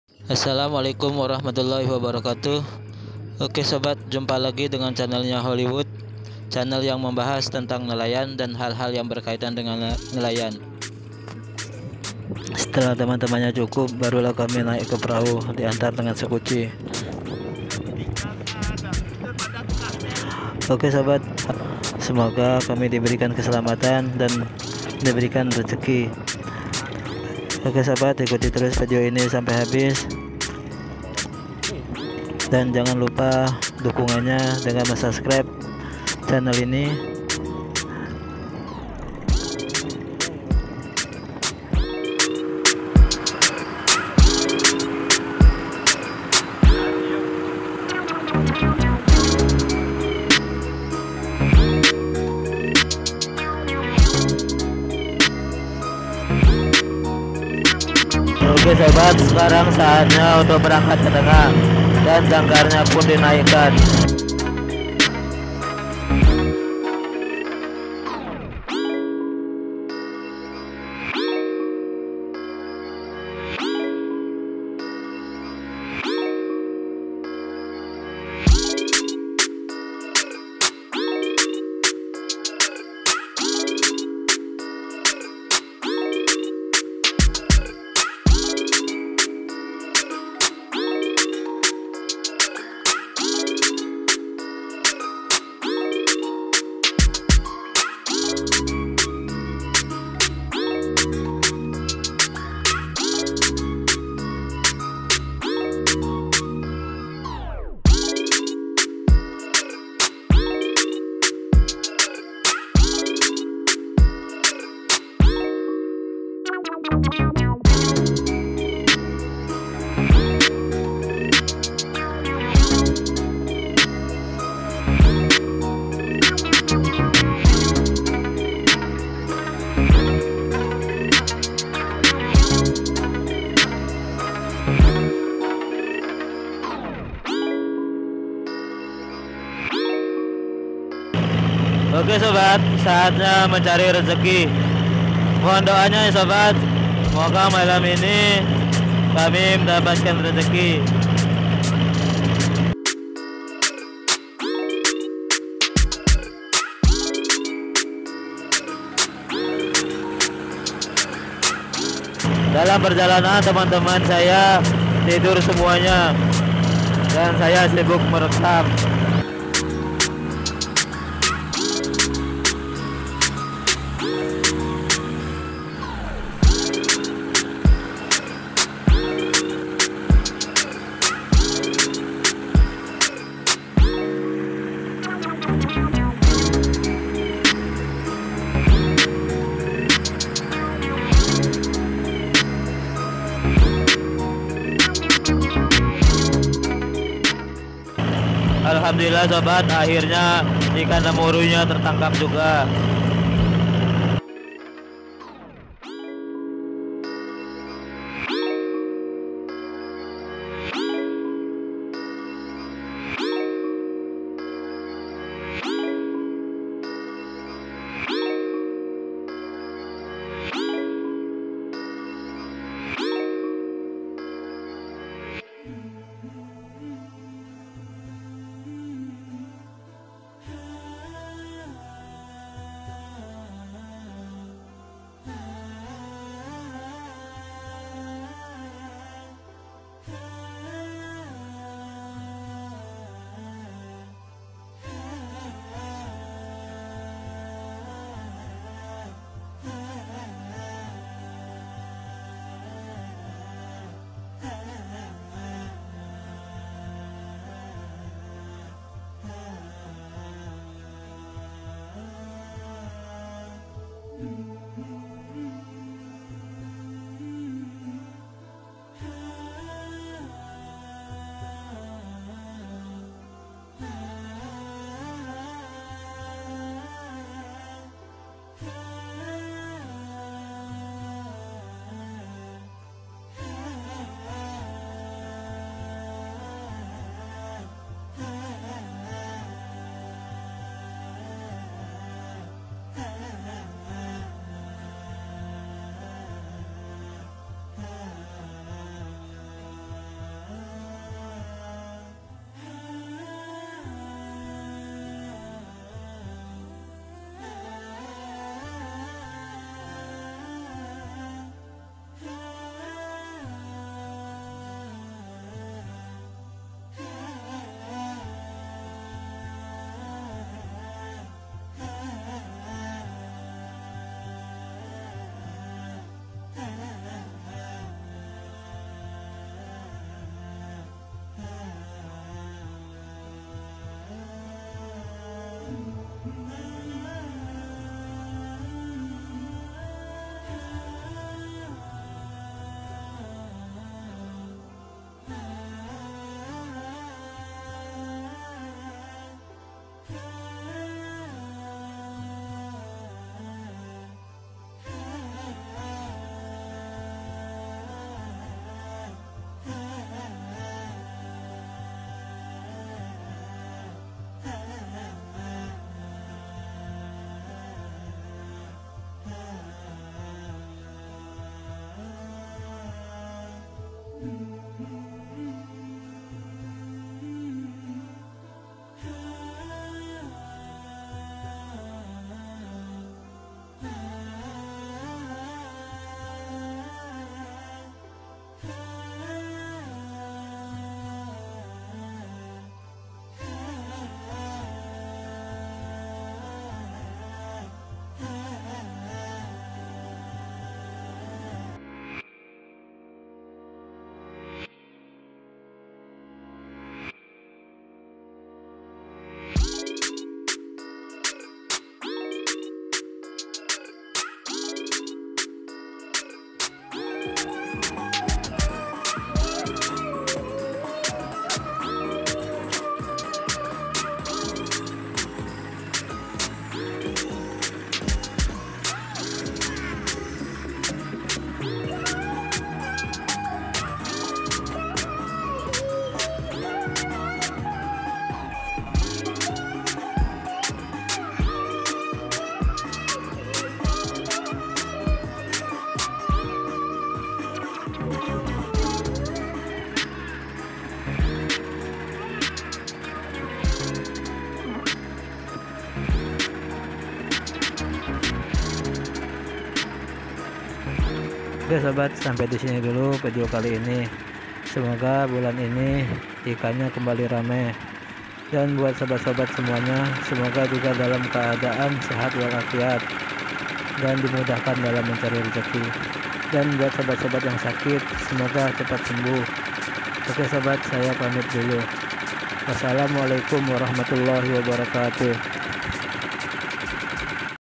Hari pertama kerja nelayan pengambengan dapat ikan lemuru | perahu bunga krantil
Hari pertama kerja Nelayan pengambengan dapat ikan lemuru dan semoga hari hari selanjutnya ikan nya tetap ada agar kami tetap semangat mencari nya.